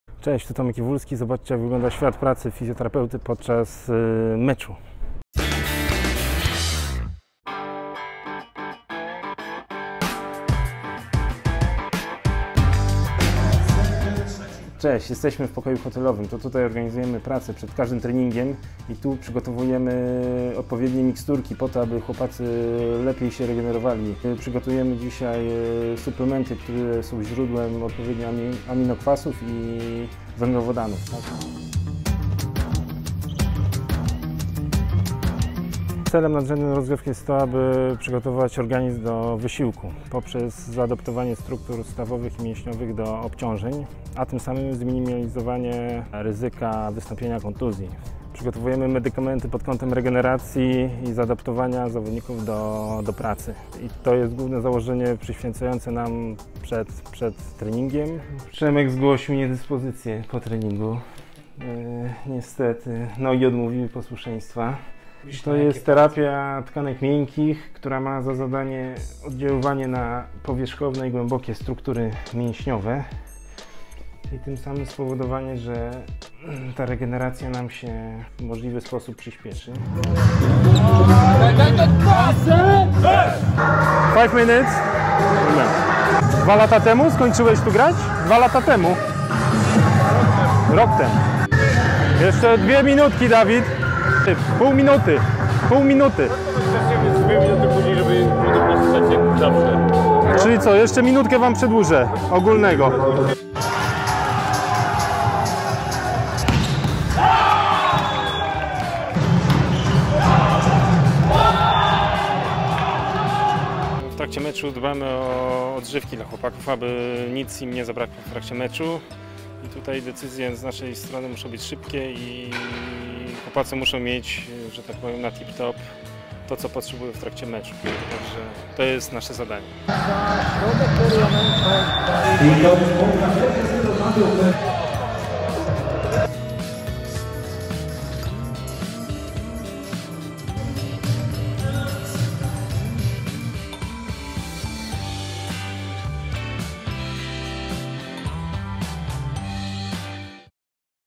AZS TV
Zobacz świat fizjo podczas wyjazdu do Bełchatowa 💪  Jakie sumplementy przygotowuje Tomek Iwulski przed treningiem? Kto po zajęciach przychodzi do Tomka na masaż? 🆘  Co przekazuje do zawodników Tomek podczas przedmeczowej rozgrzewki? 🏐  Zobacz to w najnowszym AZS TV 🎥